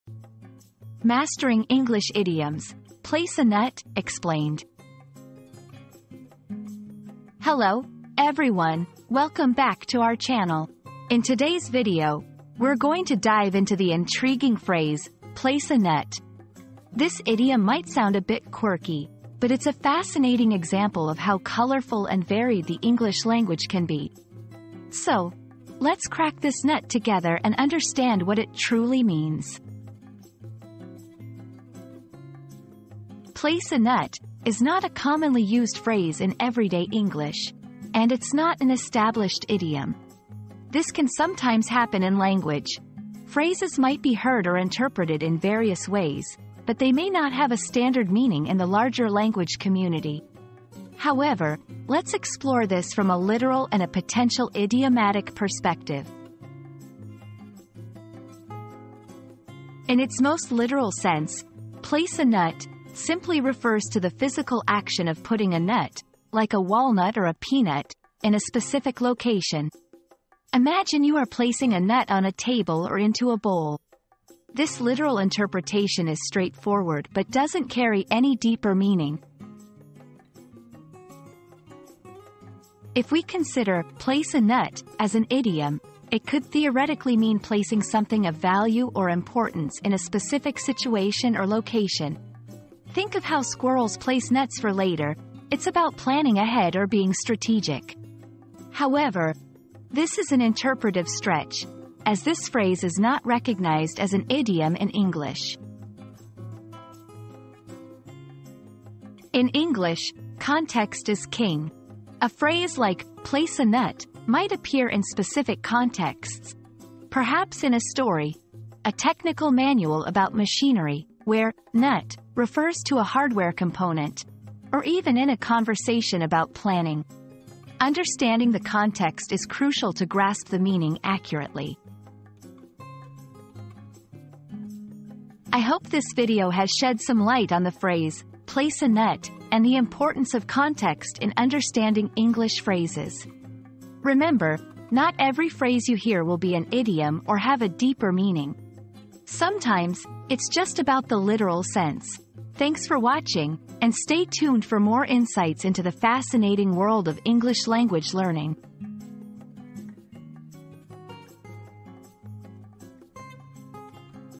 Mastering English Idioms: "Place a Nut" Explained
Unlocking English Idioms: Demystifying "Place a Nut" • Discover the hidden meaning behind the popular English idiom "Place a Nut" and learn how to use it in everyday conversations. Join us as we delve into the origins and usage of this intriguing expression, providing valuable insights and practical examples along the way. Enhance your English language skills by mastering idioms like a pro!

00:00 • Introduction - Mastering English Idioms: "Place a Nut" Explained
00:32 • Understanding the Phrase
00:59 • Literal Meaning
01:22 • Possible Idiomatic Interpretation
01:48 • Importance of Context